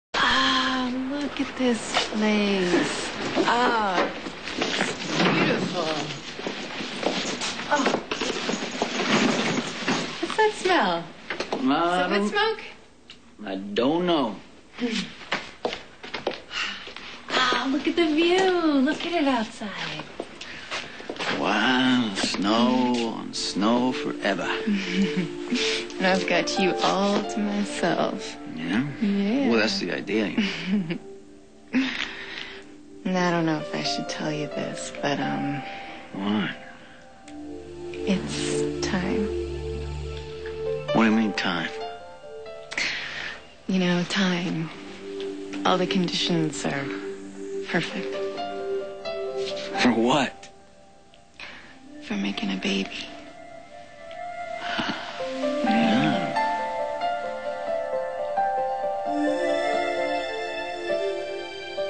Carson 02/04/2003 part 2
carson arrives at the Cabin. Carly wants them to start making their baby